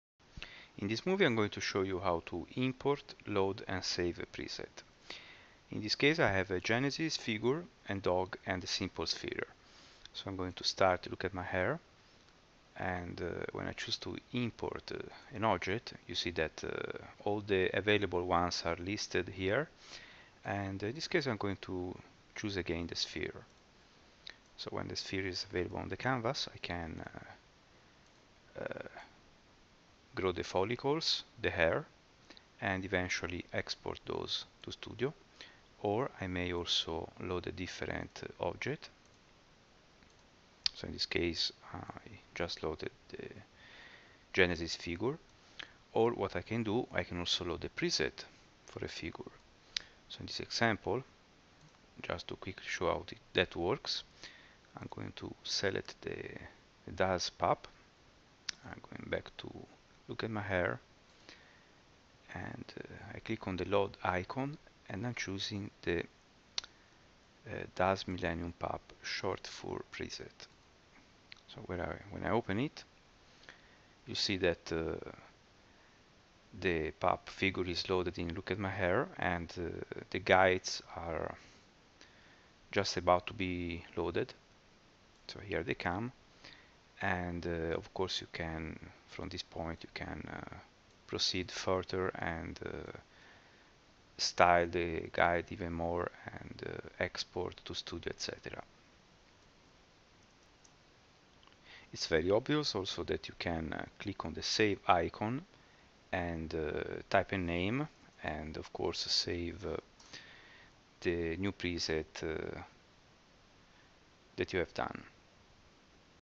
Import, load and save
A quick overview on importing object on canvas, saving and loading presets.